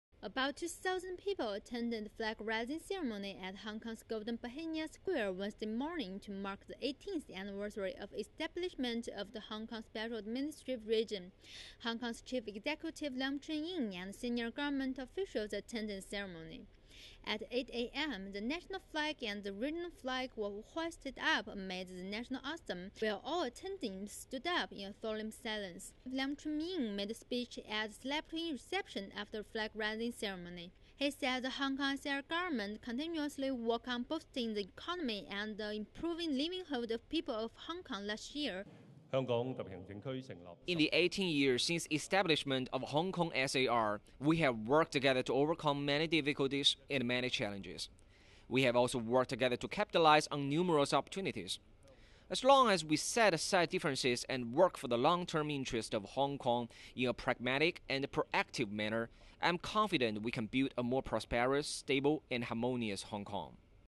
Thousands observe flag-raising ceremony for HK's 18th anniversary
About 2,000 people attended a flag-raising ceremony at Hong Kong's Golden Bauhinia Square Sunday morning to mark the 18th anniversary of the establishment of the Hong Kong Special Administrative Region.